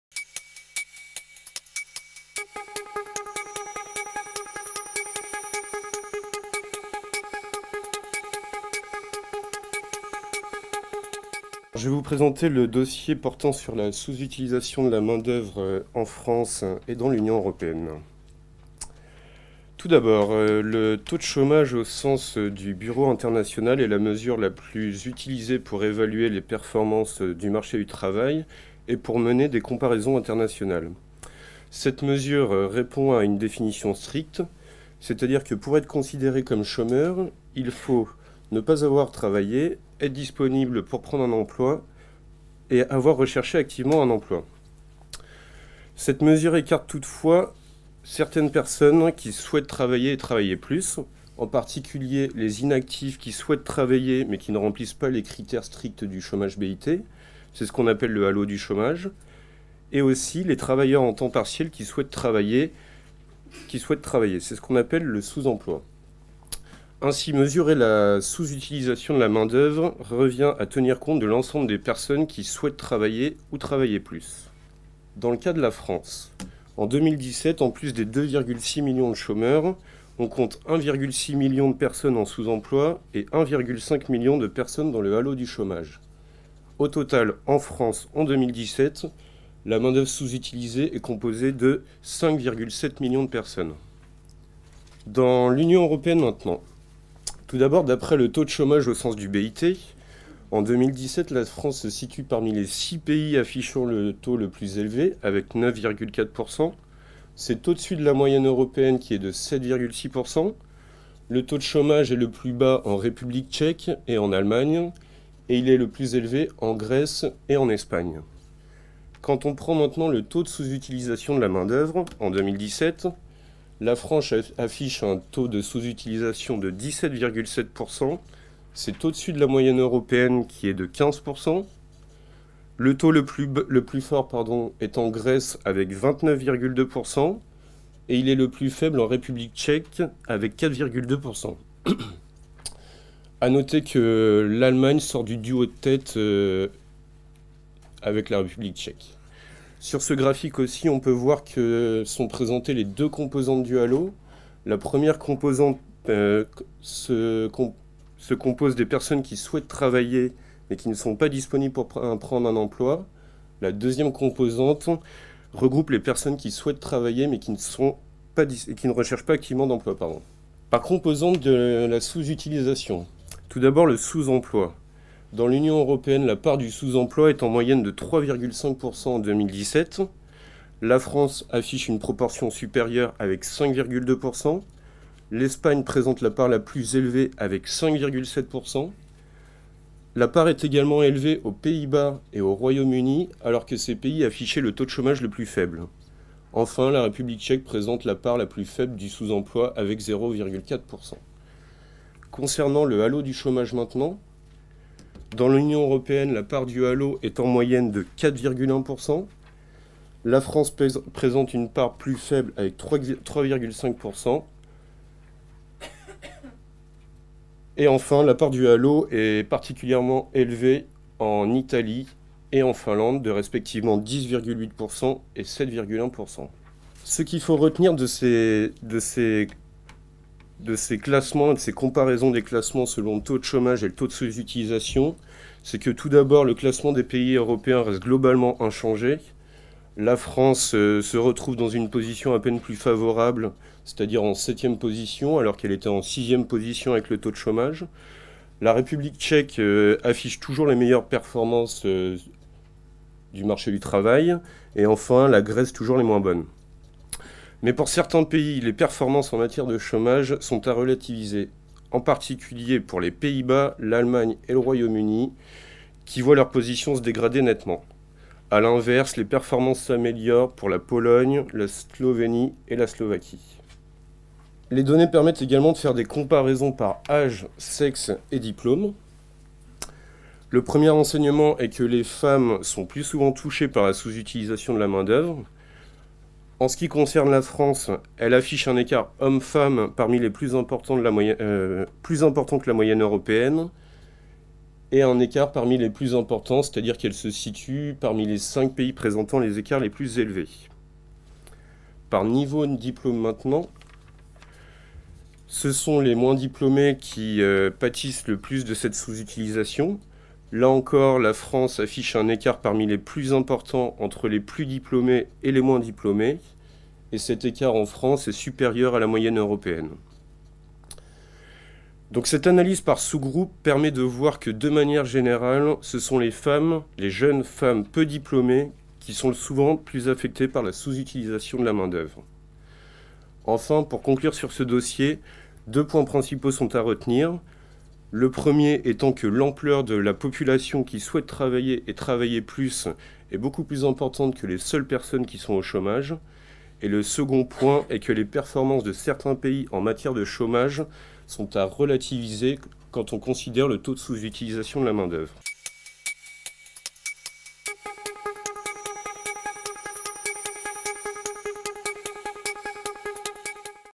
La France dans l'Union européenne
Main d'oeuvre sous utilisée : quelle ampleur dans l'Union européenne ?
Extrait de la conférence de presse du 16 avril 2019
@insee avril 2019, Production Objectif IMAGES, Réalisation : Pascal TORRE